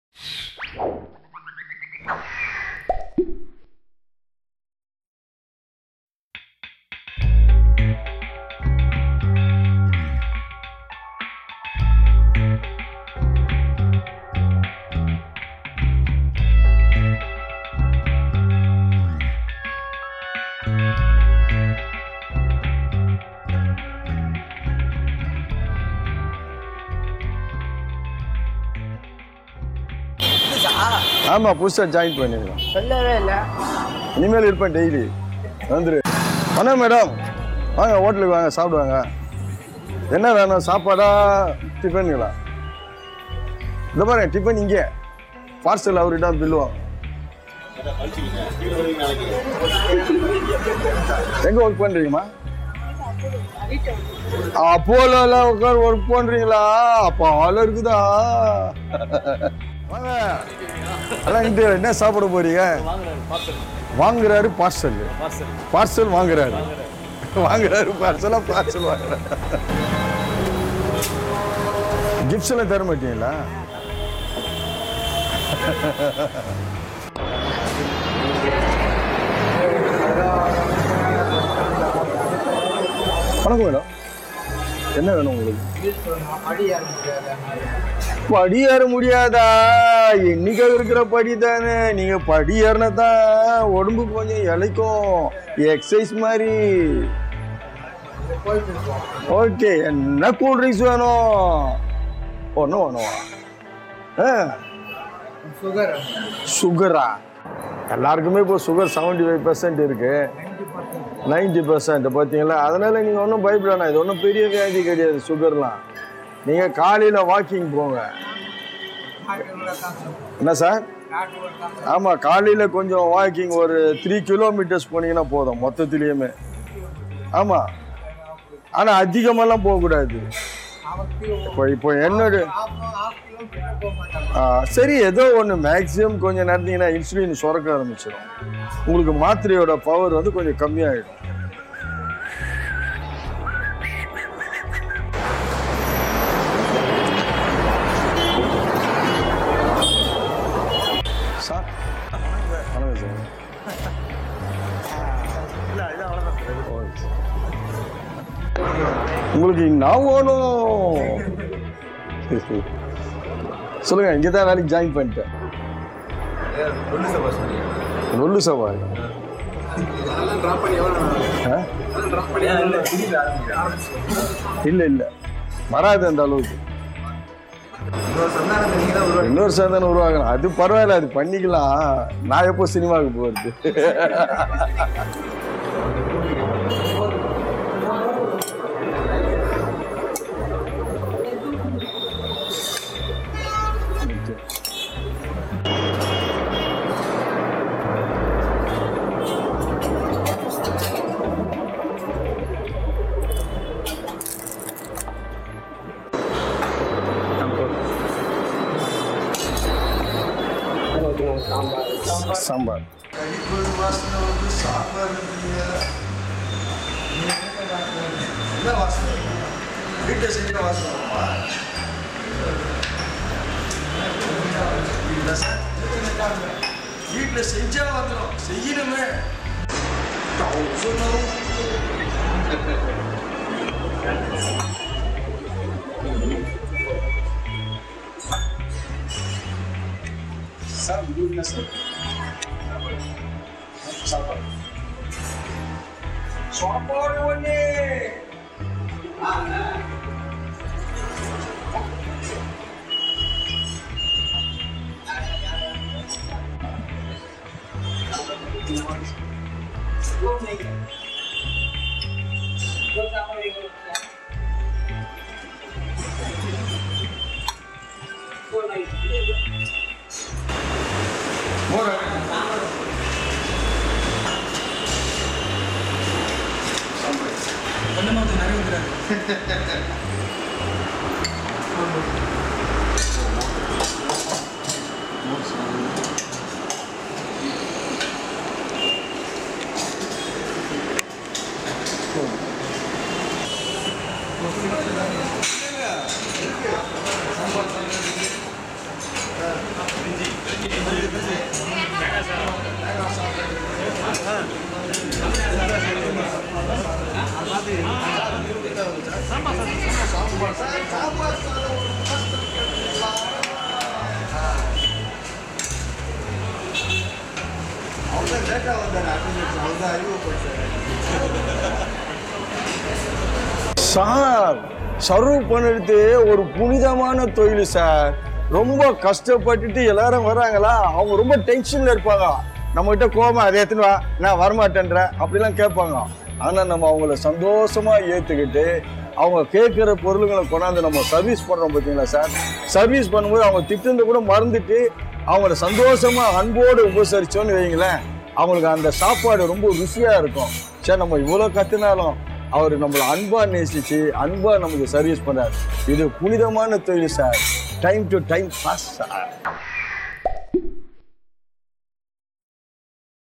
Lollu saba Manohar in hotel | One day avatar
ஒருநாள் அவதாரில் இந்த வாரம் ‘லொள்ளுசபா’ மனோகர்! சென்னை ‘நீலாபவன்’ ஹோட்டலில் சர்வர் ஆக்கினோம். நம்ம மனோகரின் அஅஅ...திரடி இதோ ஓ ஓ ஓ ஓ ஓ ஓ..!

ஒருநாள் அவதார் - மனோகர்